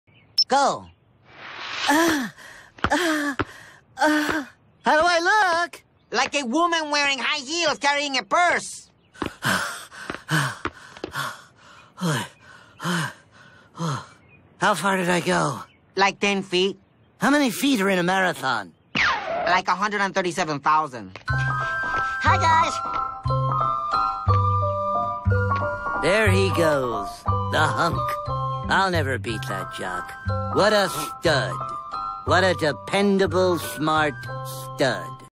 Daffy and Porky running
Daffy: How do I look?

Speedy: Like a woman waring high heels carrying a purse

💙Stay Safe!💙

🏃8/21/21🏃